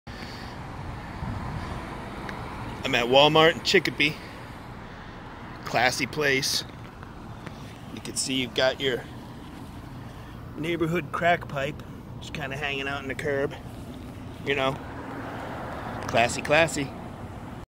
Crack pipes in the Walmart parking lot classy
Waiting for the grocery order and the parking lot is a cesspool for addicts.  Had the usual panhandler asking me for money with the usual excuses